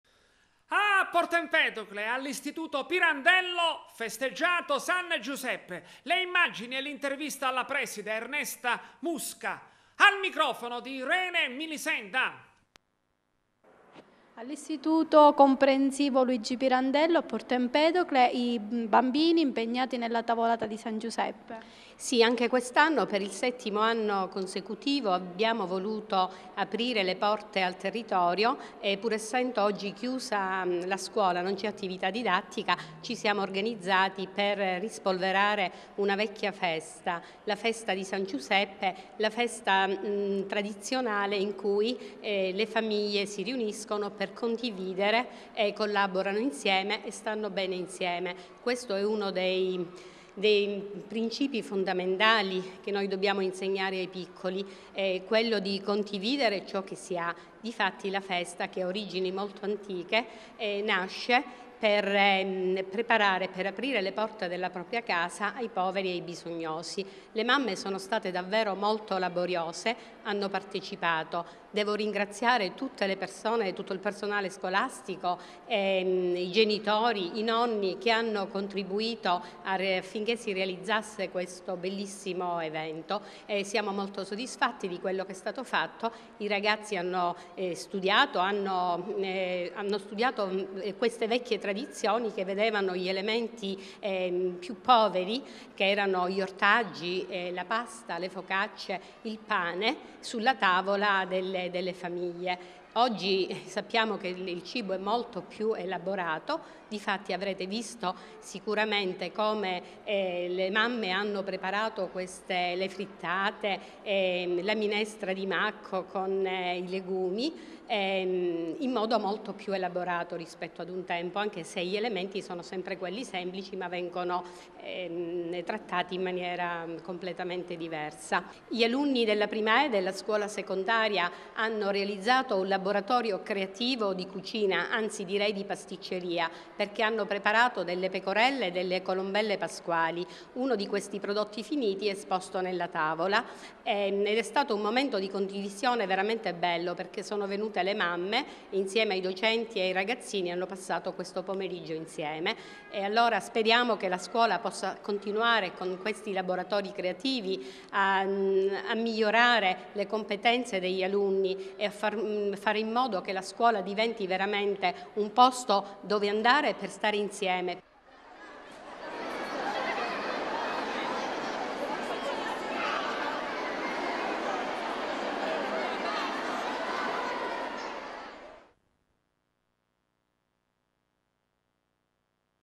Teleacras - San Giuseppe alla "Pirandello" a Porto Empedocle
A Porto Empedocle, alla scuola media “Pirandello”, festeggiato San Giuseppe. Le immagini e l’ intervista alla preside, Ernesta Musca, al microfono di Irene Milisenda.